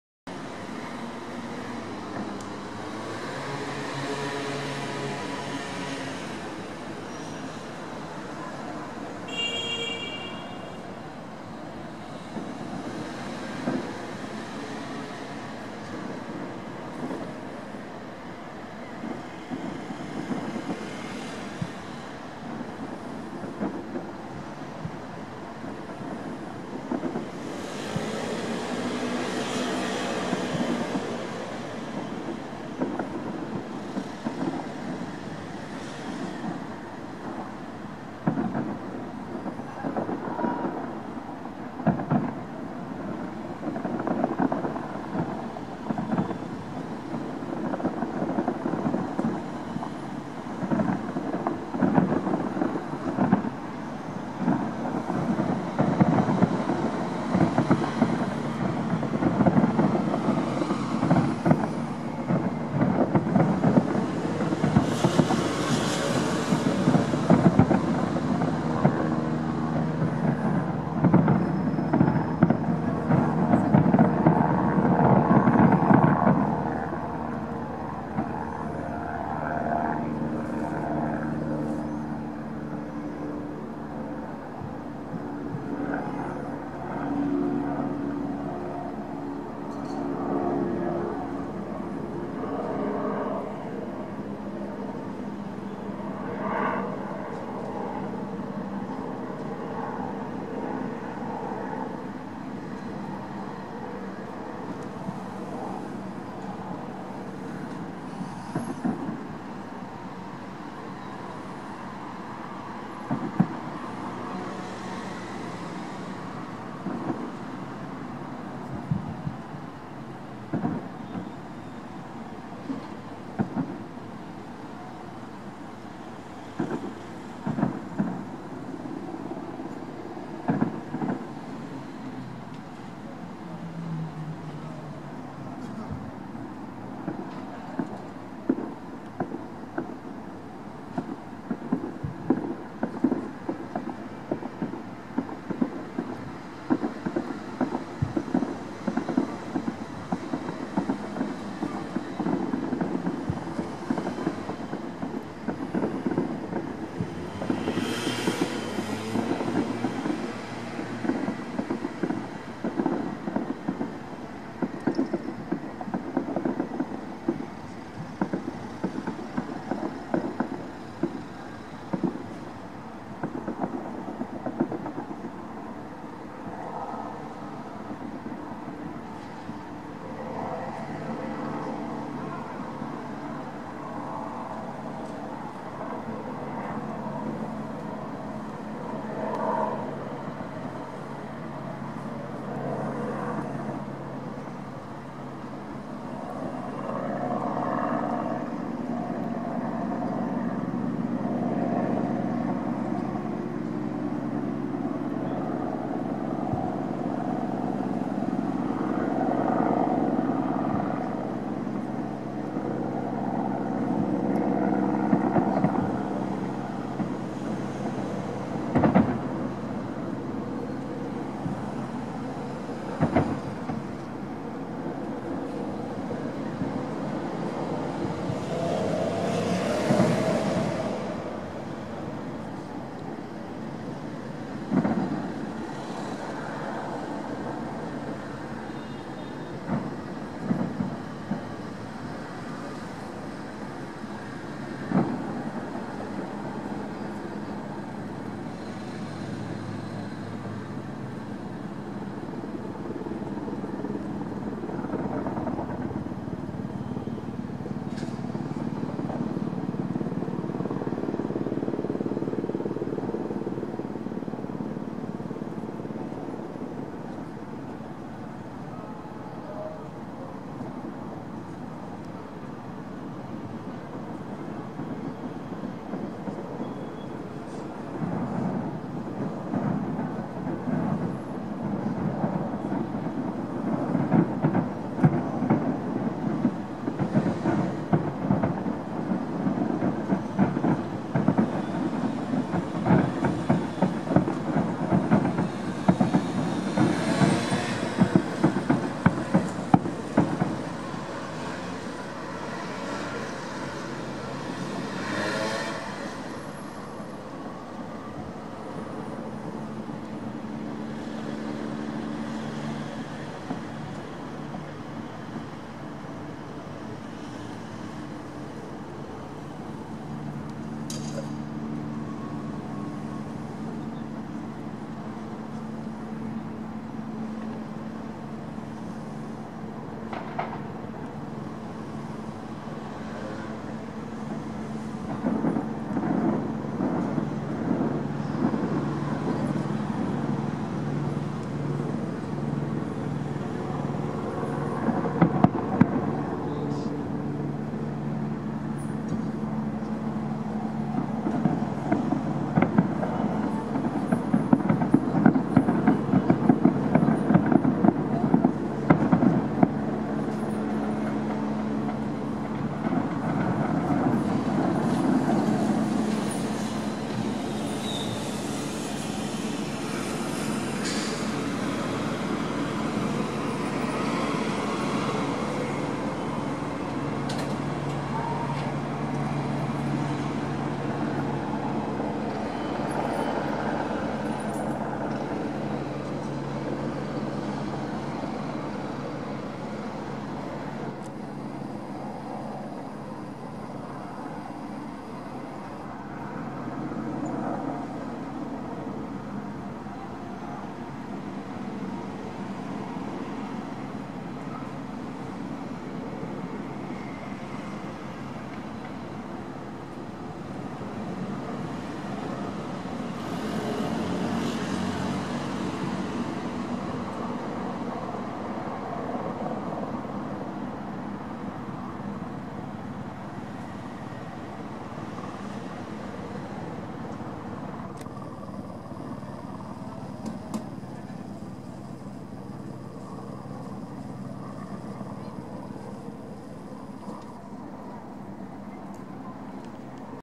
Paris - Feux d'Artifice-14 Juillet 2015 - Vu De Neuilly Sur Seine - Bouquet Final
Malheureusement, trop loin pour entendre la musique.
Unfortunately, too far away to hear the music.